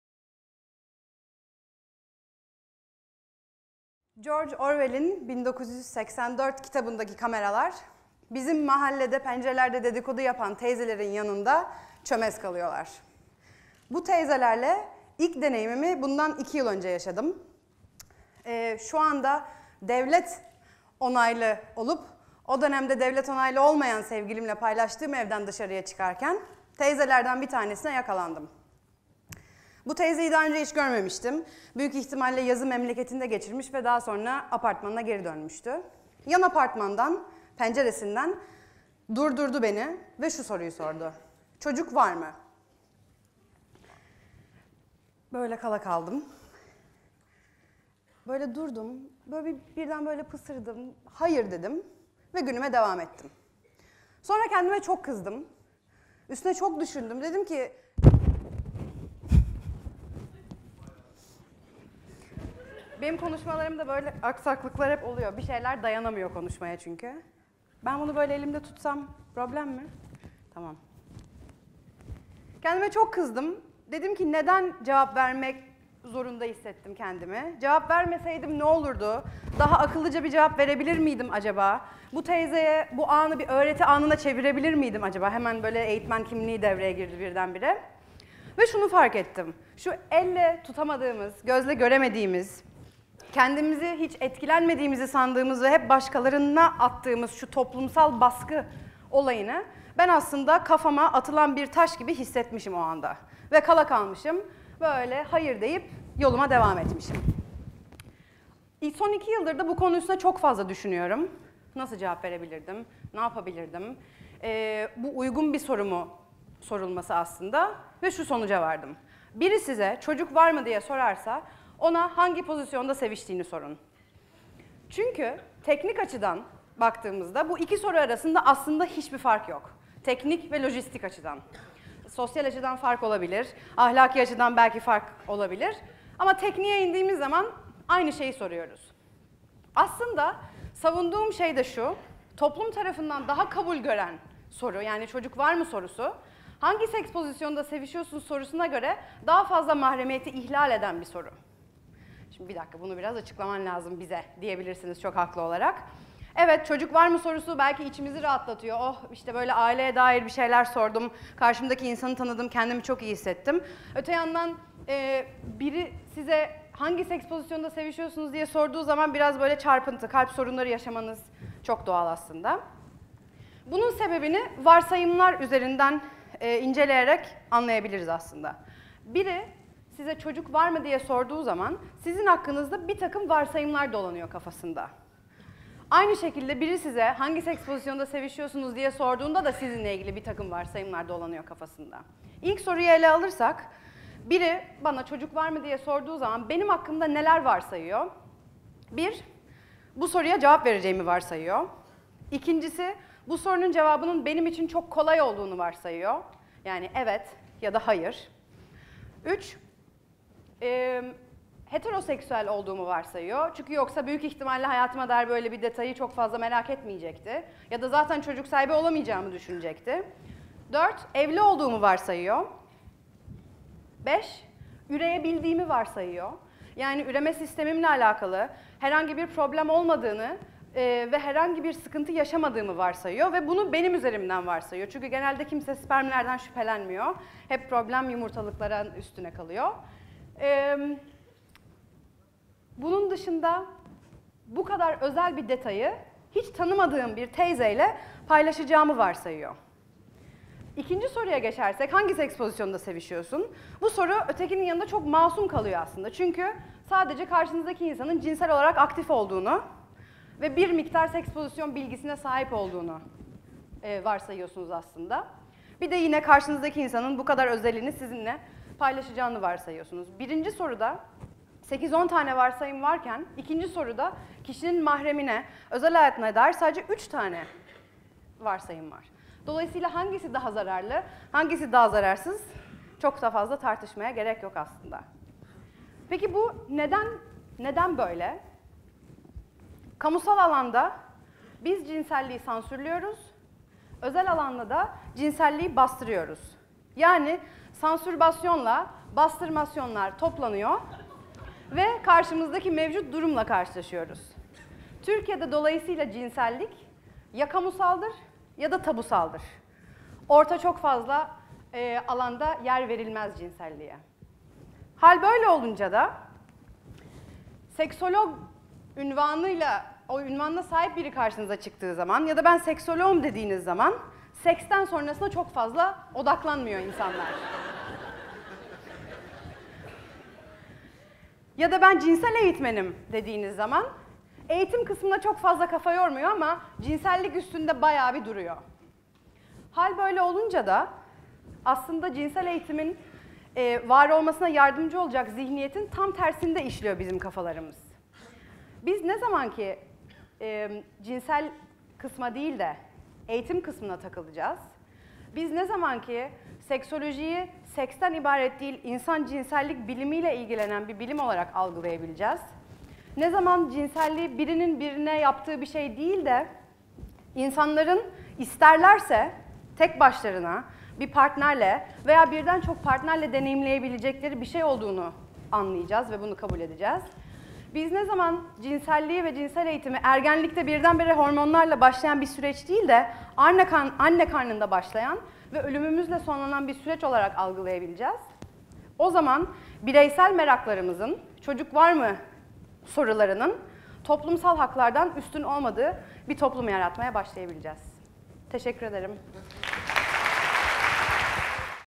Kamusal Seks, Tabusal Seks | Rayka Kumru | TEDxİKÜ
Seksolog Rayka Kumru toplumun cinselliğe bakış açısındaki ikilemi; mahremiyet, meraklar ve haklar bağlamında değerlendiriyor
 Seksolog Rayka Kumru
Seksolog, kapsamlı cinsellik eğitmeni, ve danışmanı olan Rayka Kumru The University of British Columbia’da Sosyoloji ve Cinsellik Bilimleri lisans eğitimi üzerine, Avustralya’da Curtin University Halk Sağlığı Bölümü’nde Seksoloji Yüksek Lisansı’nı tamamlamış ve tezini Türkiye’de 18-24 yaş arası gençlerin korunma yöntemleri hakkındaki tutum ve deneyimleri üzerine yazmıştır. Beni de Bedenimi de Seviyorum çocuk ve ebeveyn kitap setinin yazarı olan Rayka Kumru Yıldız Teknik Üniversitesi ve İstanbul Kültür Üniversitesi’nde öğretim görevlisi olarak cinsel sağlık eğitimi, insan cinselliği ve toplumsal cinsiyet dersleri vermiştir. Rayka Kumru Avrupa Seksoloji Federasyonu Genç Komitesi üyesi ve araştırmacısı, Dünya Cinsel Sağlık Derneği üyesidir. This talk was given at a TEDx event using the TED conference format but independently organized by a local community.